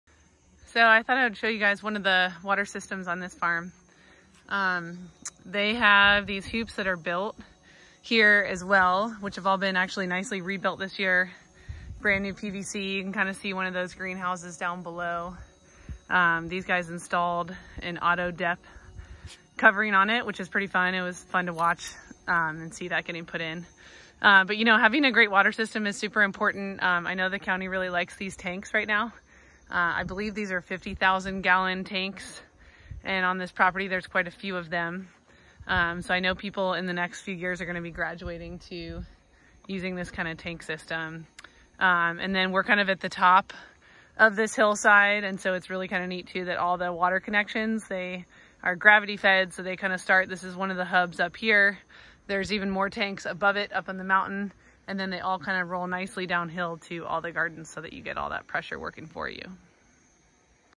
Emerald Healing/ S. Face Rd Water system
This is the way the county likes the water systems to look.